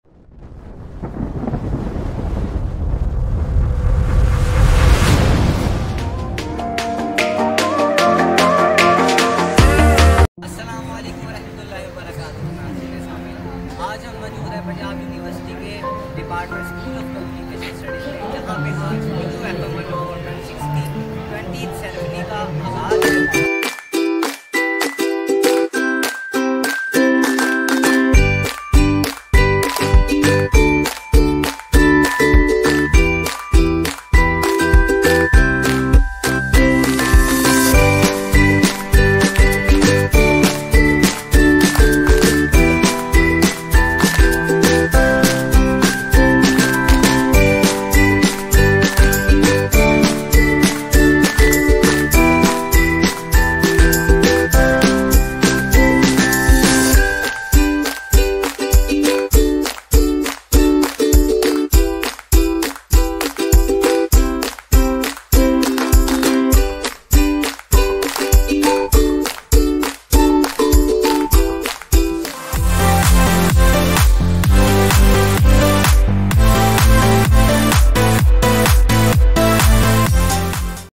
PUFM 104.6 || 20th ANNIVERSARY celebration || vlog by : MEER HAMZA ABBAS
University of Punjab || SCHOOL OF COMMUNICATION STUDIES || there are complete 20 years .. Congratulations PUFM 104.6
Here i was share with you some little vlog of PUNJAB UNIVERSITY FM 104.6 or some views of department of communication studies
Share my YouTube channel /